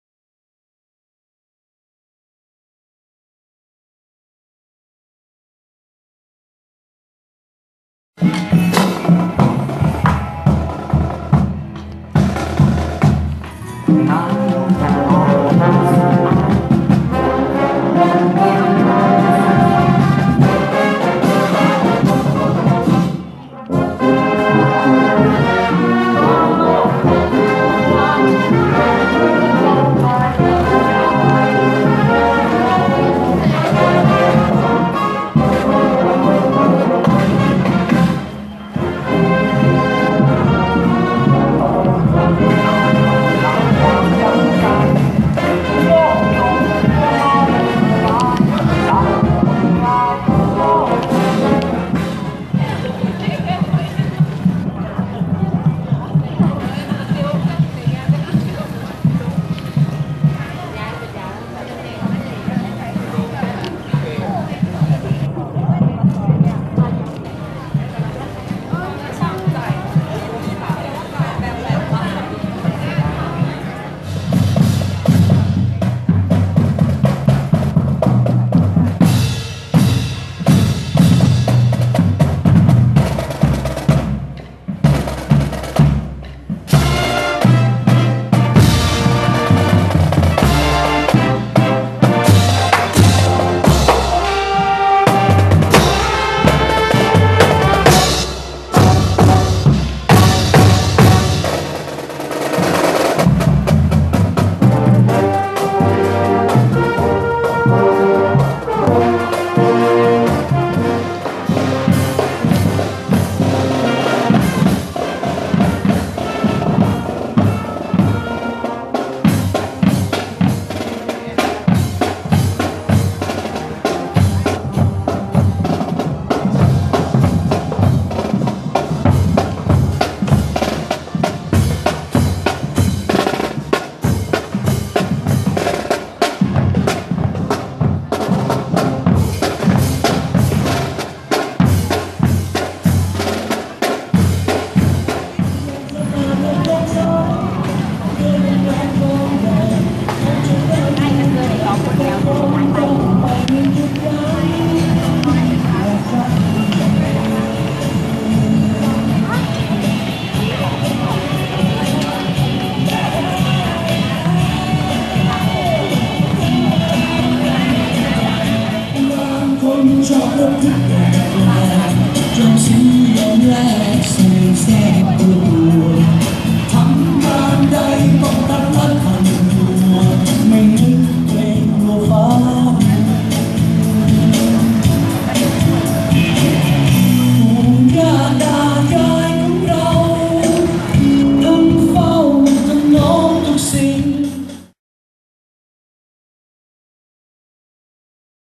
ANTI CORRUPTION
La journee de la lutte contre la corruption le 9 decembre donne lieu a des defiles des ecoles accompagnees de leurs fanfares. Ici a Lamphun
Est-ce que c'est suffisant?